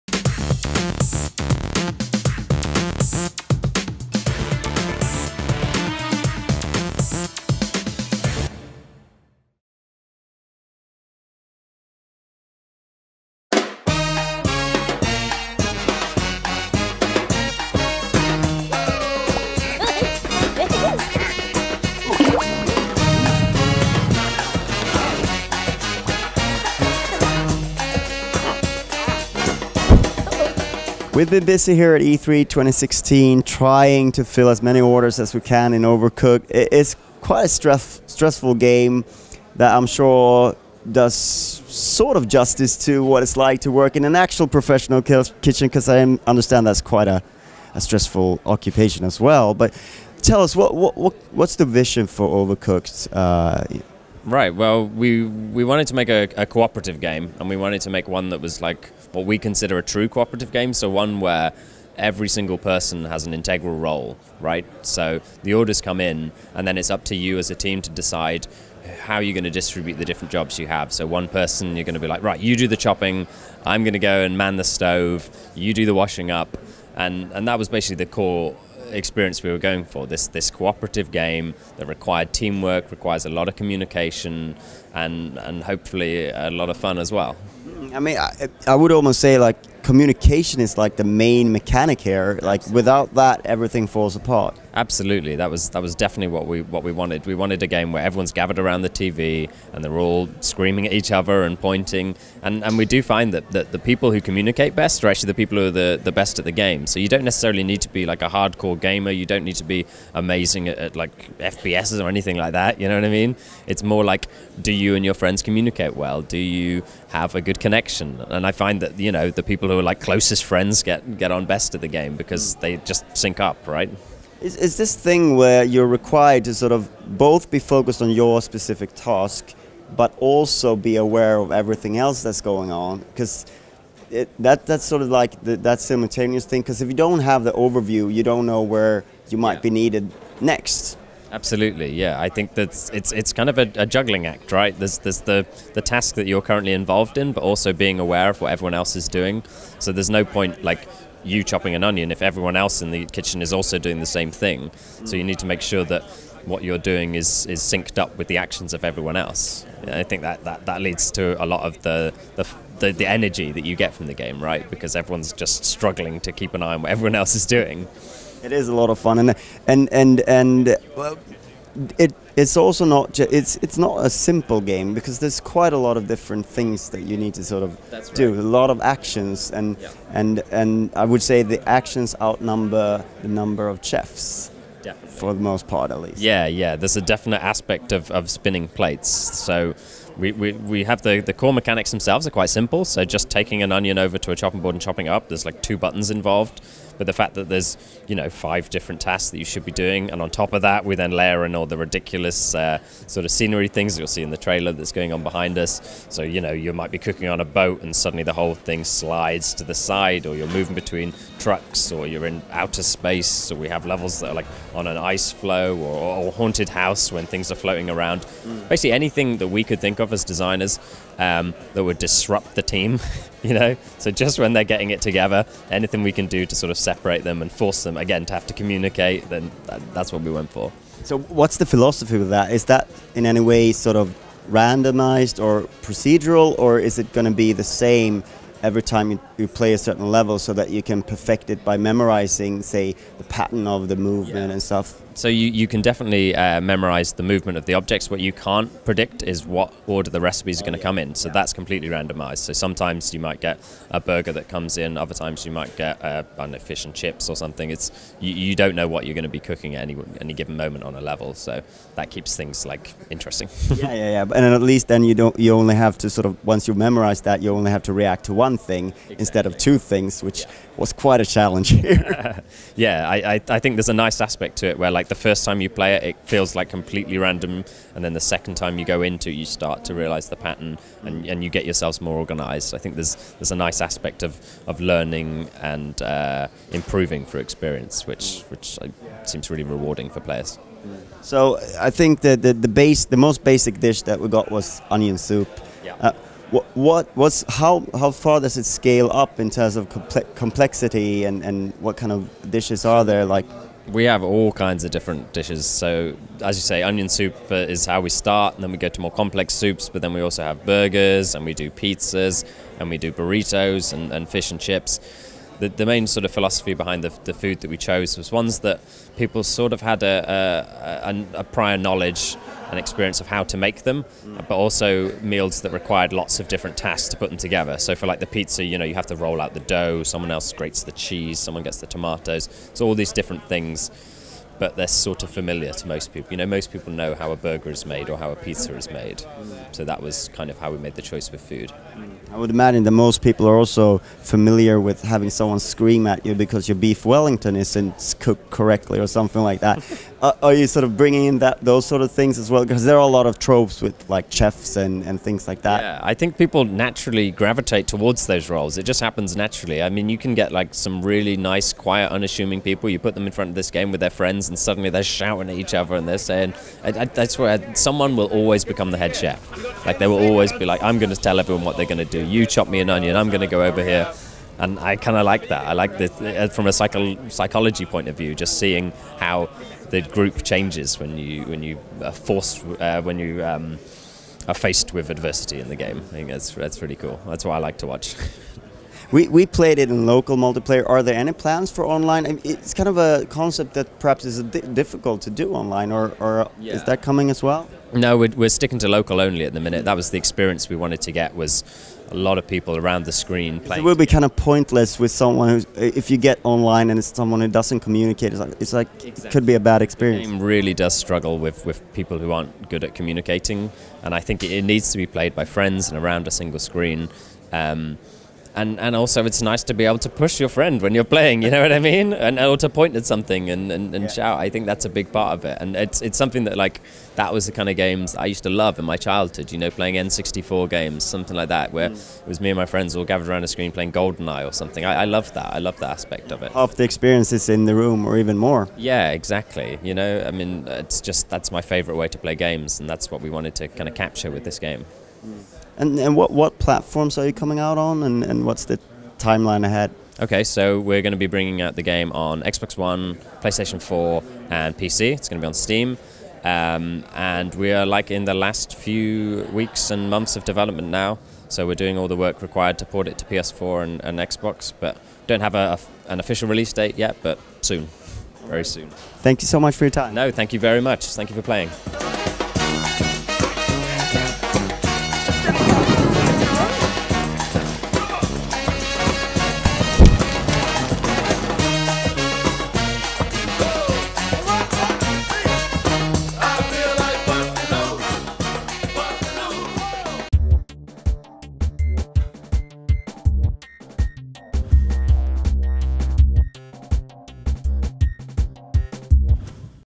Overcooked - Phil Duncan Interview
We talked to head chef Phil Duncan about the chaotic couch co-op title, Overcooked.